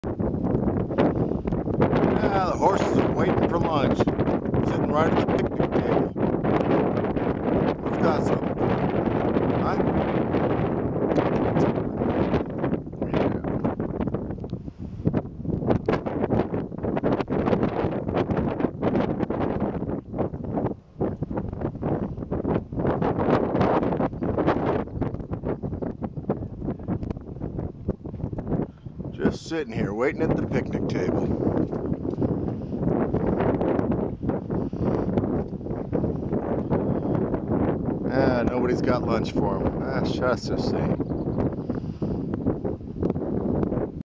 HORSE PICNIC
Horses Shading themselves at the picnic table.